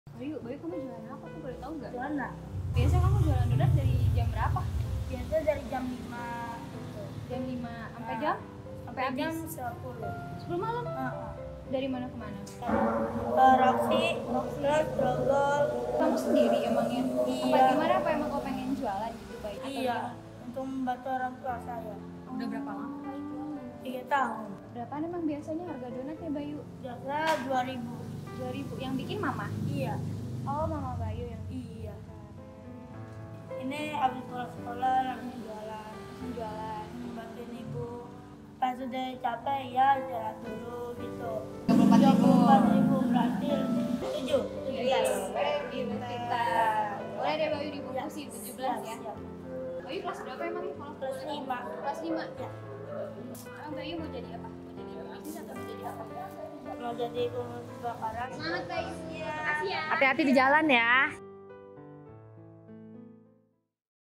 Bayu, anak yang menanggung beban besar di pundaknya yang kecil
walau umurnya masih sangat muda, keinginan bayu membantu orangtuanya sangatlah besar

Rumahberkat.com adalah website untuk menggalang dana dan berdonasi secara online dan transparan.